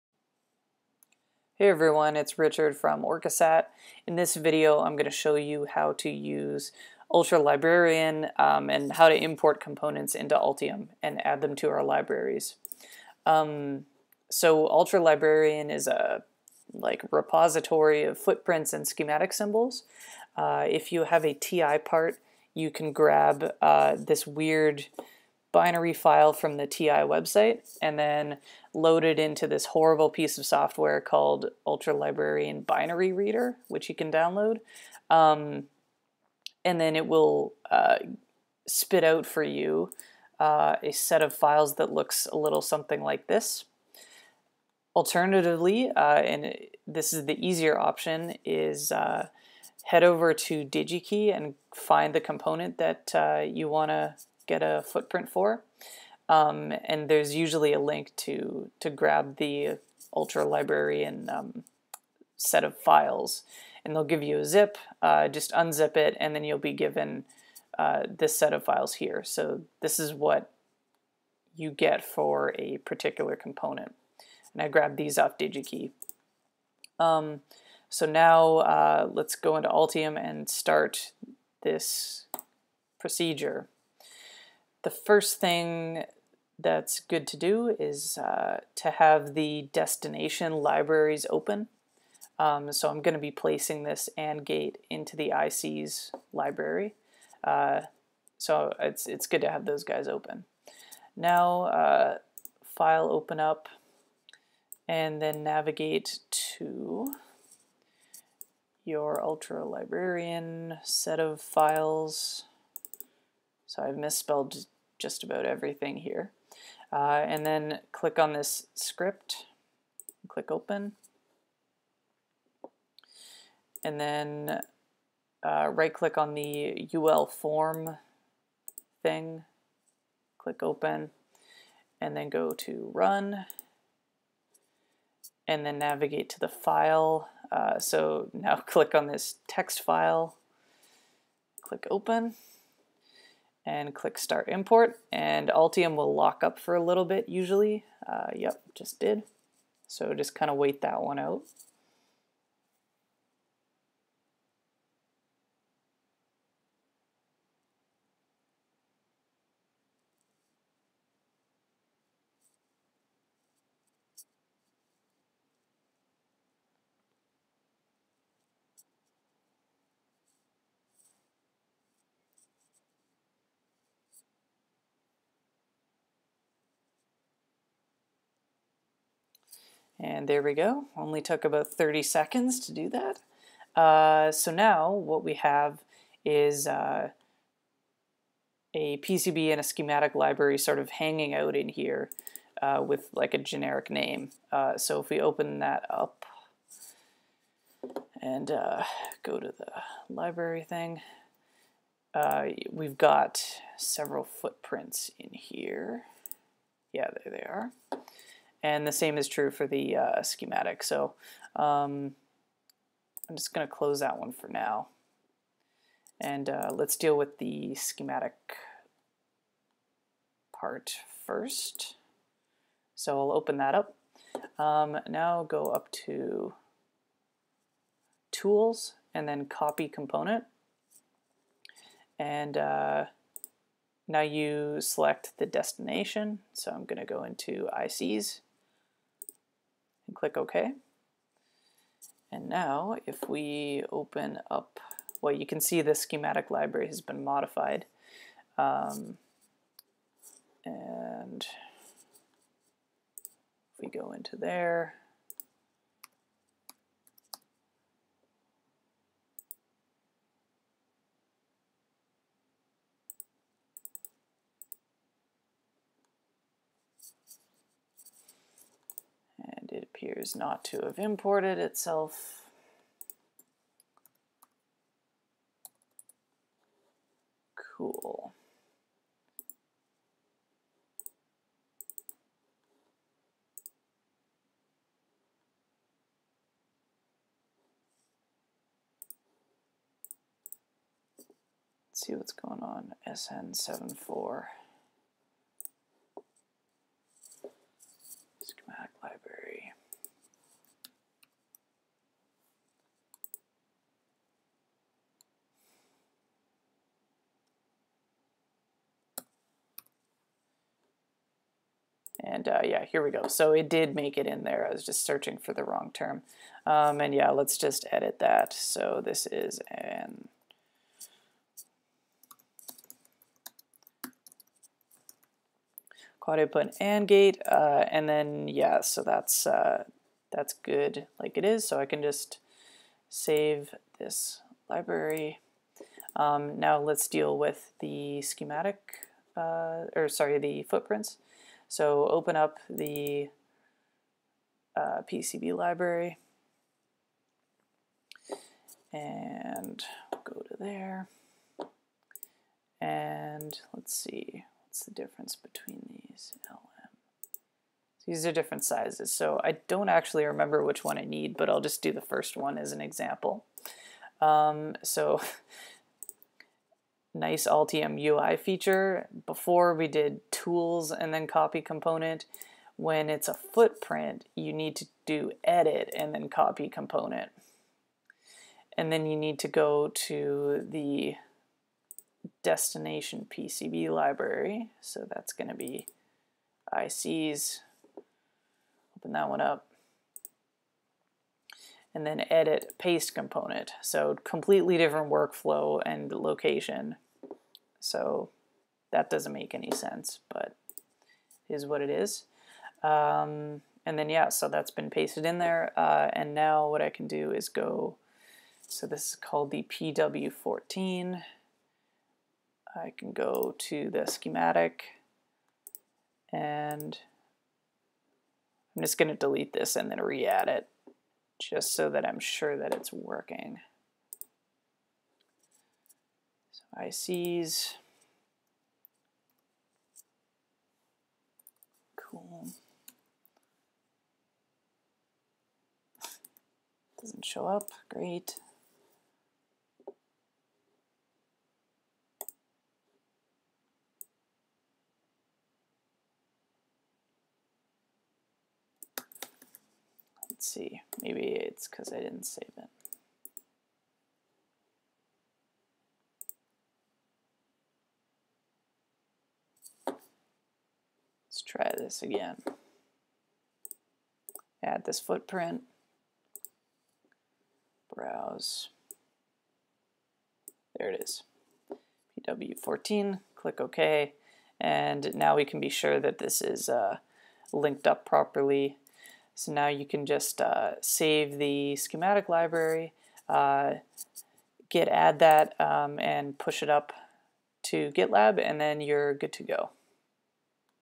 Copying Altium Components and Footprints, Ultra Librarian
How to use ultra librarian, where to find components, copying components between libraries.